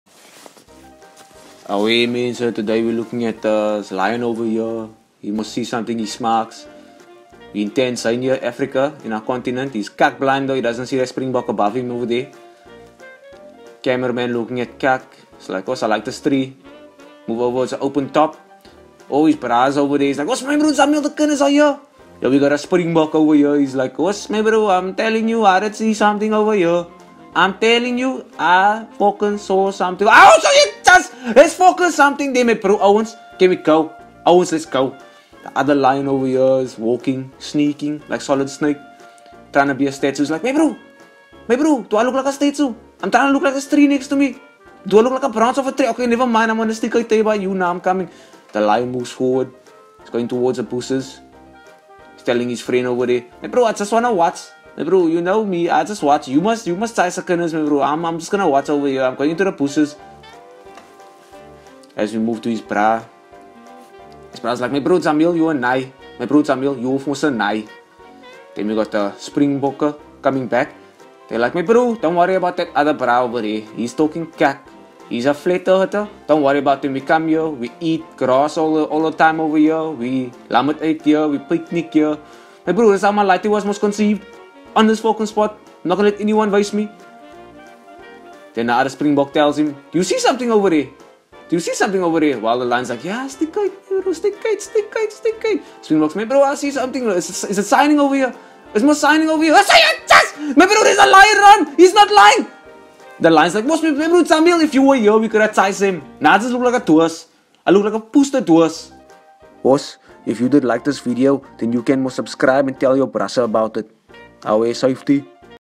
South African reviews a Lion hunting Springbok
Comedy video about a South African reacting and reviewing A lion hunting an animal in a funny and colorful fashion.